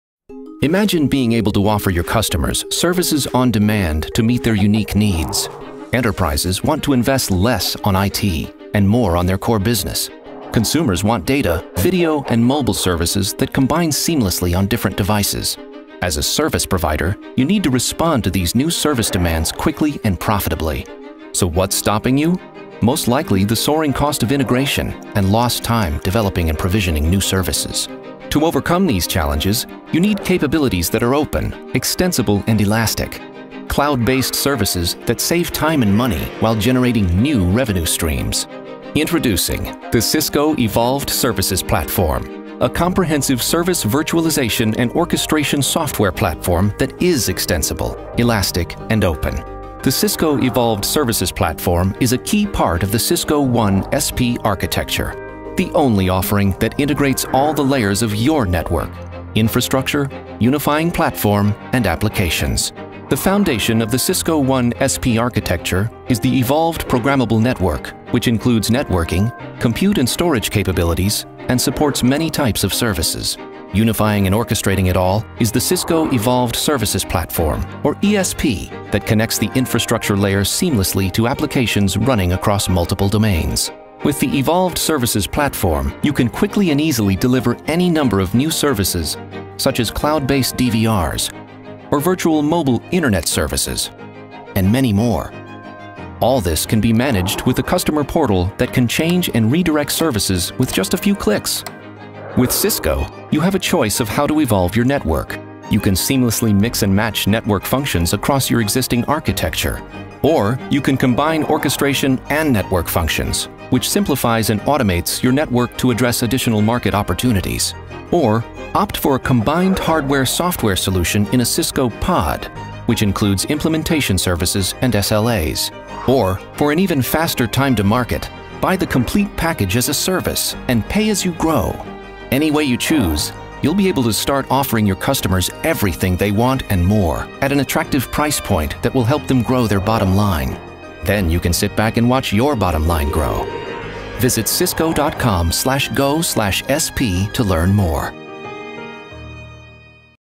What Is Cisco Evolved Services Platform (ESP)
The Cisco Evolved Services Platform (ESP) is a comprehensive virtualization and orchestration software platform that creates, automates and provisions services in real time, across compute, storage and network functions, to deliver desired business outcomes for applications running across multiple domains and facilitates the shift to new business models